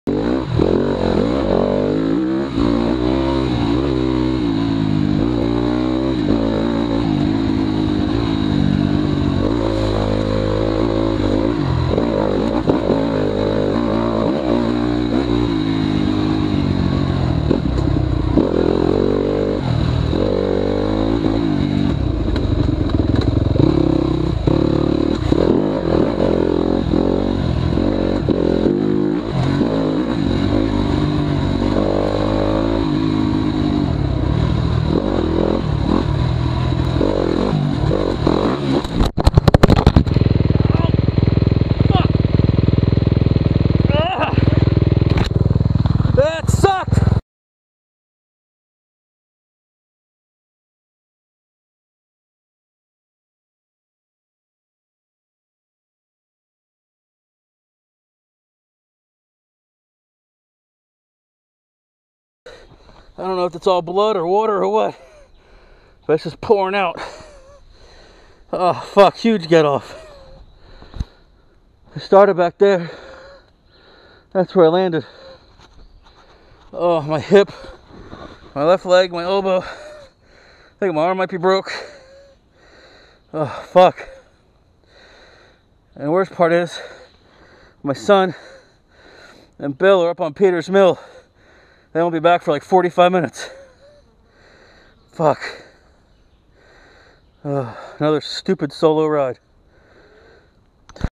taskers gap crash 5/21/2017
Took a bad line and tried to power out of it and monster truck a rock. didn't work.